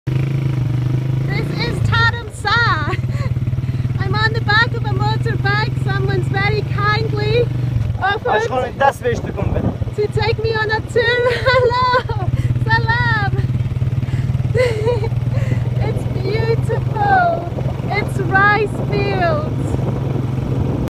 On the back of a motorbike in Iran
Spent 5 weeks back packing Iran in May 2015 - here's me on the back of a motorbike ride through the rice fields of Taromsar in Northern Iran.